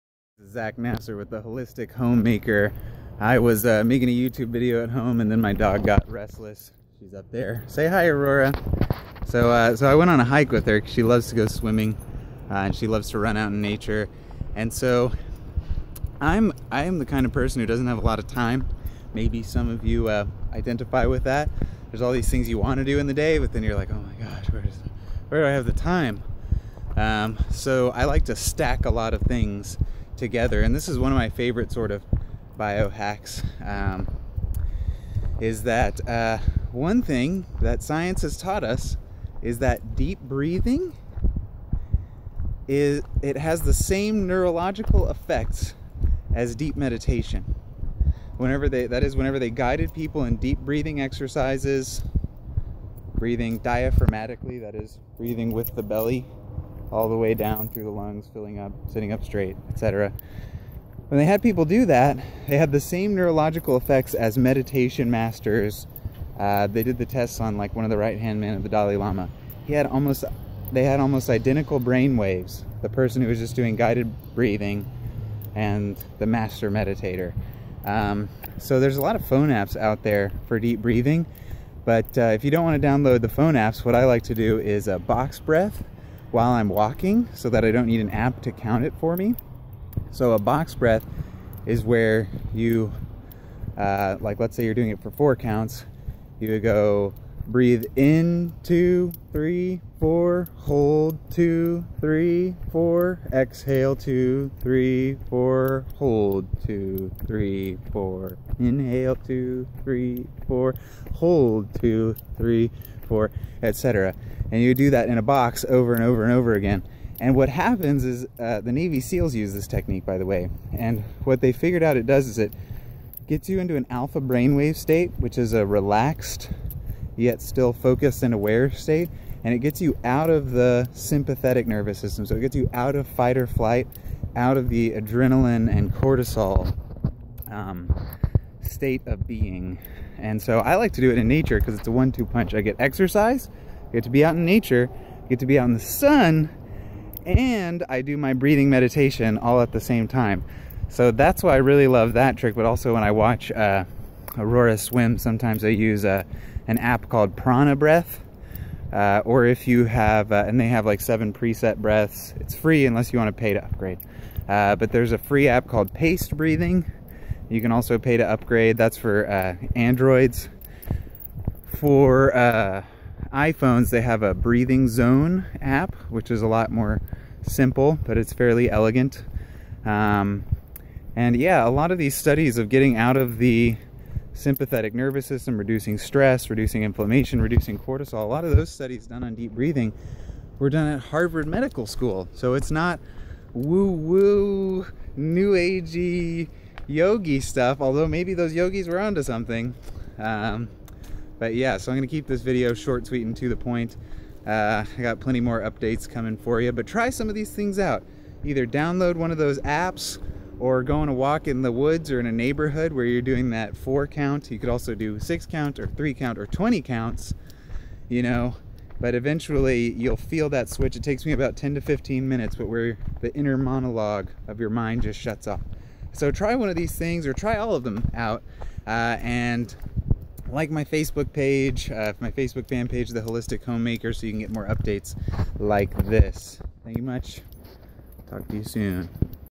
If you have trouble meditating then watch this video!
Did you know that meditation and deep breathing have the same neurological effects? They do! Here are some of my favotite techniques and phone apps that you can start using today to get the benefits of meditation in your life.